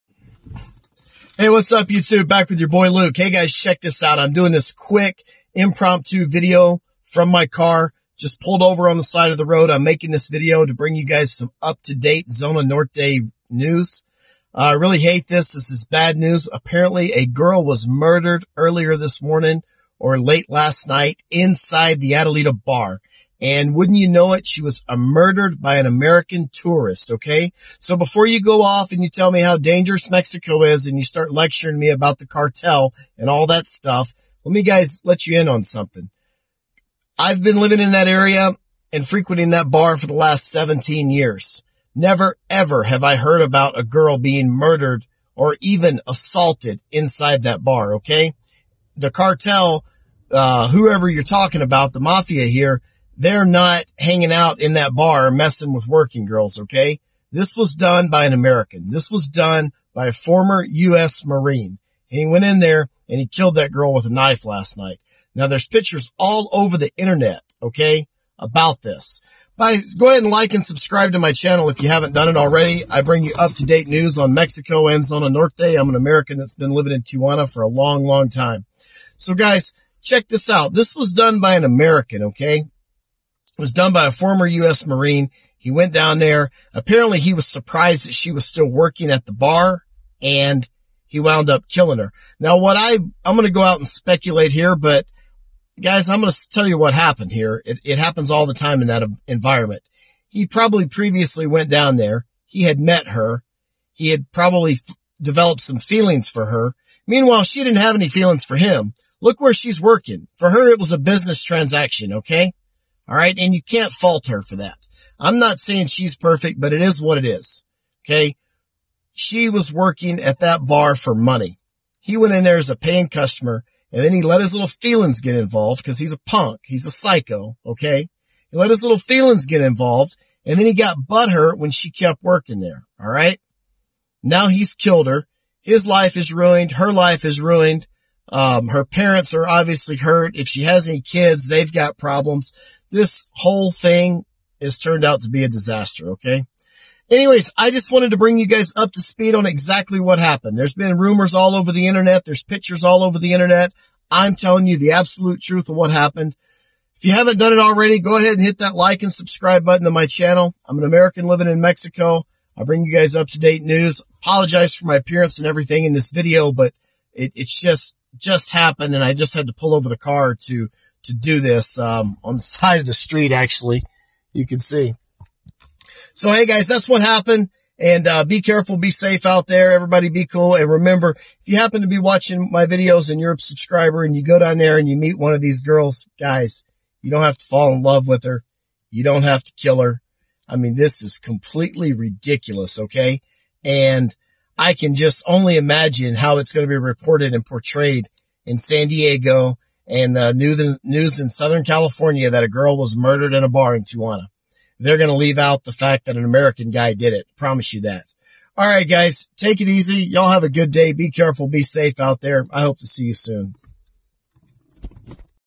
American Attacks Girl in Adelita Bar in Tijuana.
A former US Marine stabbed a girl to death in Tijuana's Red light district.